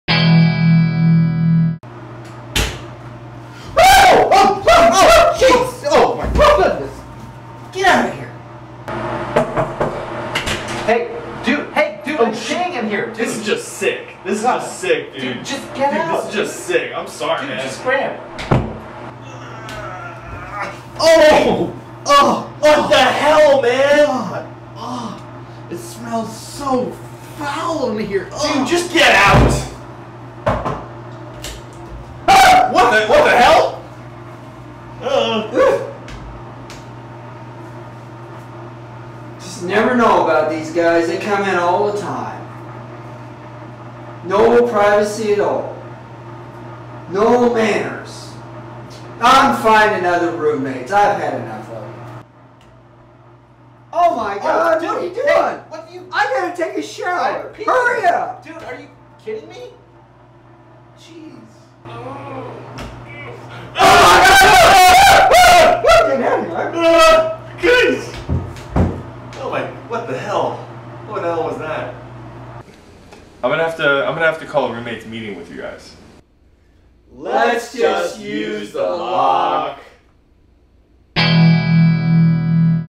Dude! Comedy: Privacy
Joe, Matthew and Travis can't seem to get any privacy.

Starring Matthew Goldin, Travis Rust, and Joe McAvoy